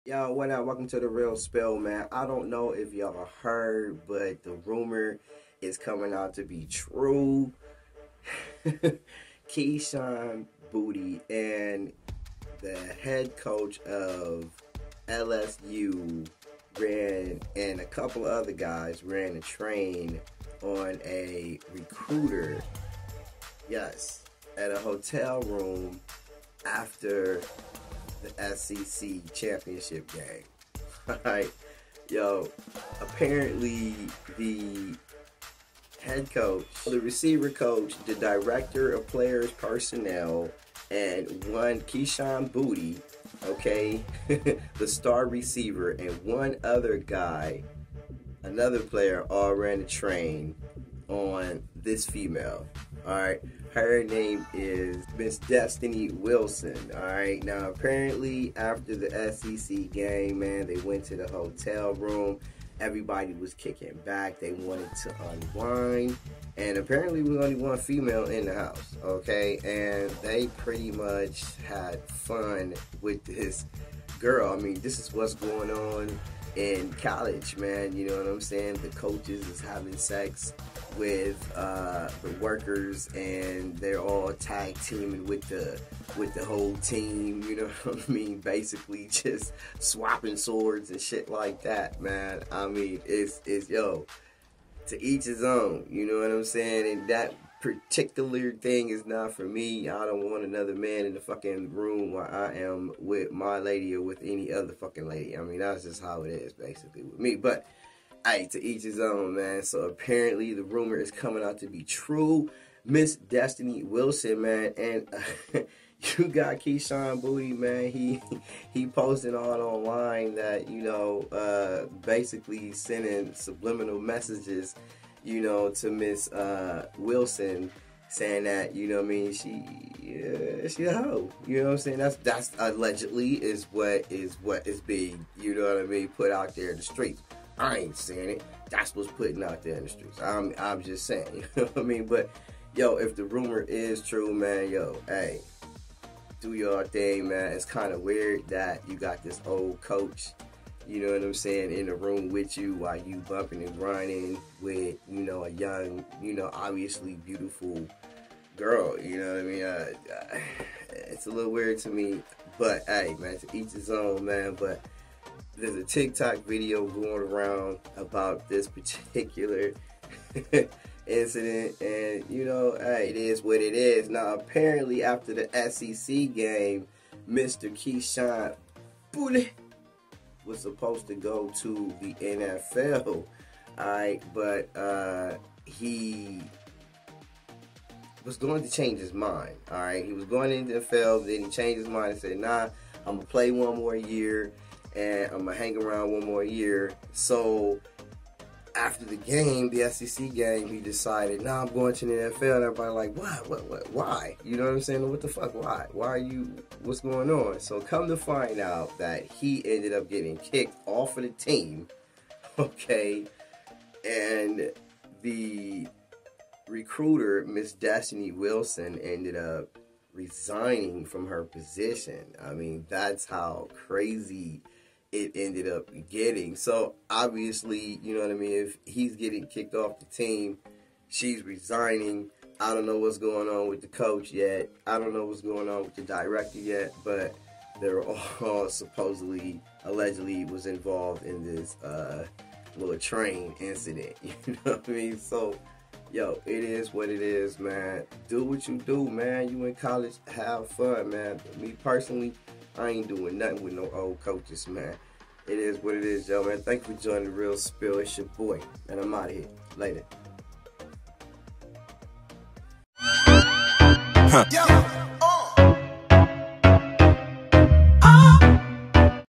LSU Coach & Player's Allegedly Ran A Train On The Recruiting Girl… Here's What Happened!
Back on Dec. 5, LSU Tigers wide receiver Kayshon Boutte announced that he would not be declaring for the NFL Draft, and that he will be returning to the program next season. With that, Boutte would also be playing in this year’s Citrus Bowl, as LSU takes on the Purdue Boilermakers. Less than a month later, days before the bowl game, Boutte had a change of heart, announcing that he is declaring for the draft after all.

This had some fans wondering what caused Boutte to change his mind. There is a rumor circulating on the internet, which we must stress has not been confirmed by the school or anyone else. Those rumors? A sex party involving members of the LSU program. a supposed message involving a rumor that there was a sex party during LSU’s stay in Atlanta ahead of the SEC Championship Game against the Georgia Bulldogs. The message said that there were at least five participants were identified, Boutte being among them. Other rumored participants included members of the coaching staff and recruiting department. The message claims that Boutte was “dismissed.” Again, these are rumors that are not confirmed yet, so take them with a grain of salt.

Boutte announced his decision to declare for the NFL Draft on Dec. 28. When speaking with the media on Sunday, LSU head coach Brian Kelly explained his decision to list Boutte as “unavailable Unavailable, as you know, means I can’t speak to some of the reasons behind it or I would have been more specific,” said Kelly,

This season, Boutte recorded 48 receptions for 538 yards and two touchdowns in 11 games. In his three years with the program, Boutte caught 131 passes for 1,782 yards and 16 touchdowns in 27 games.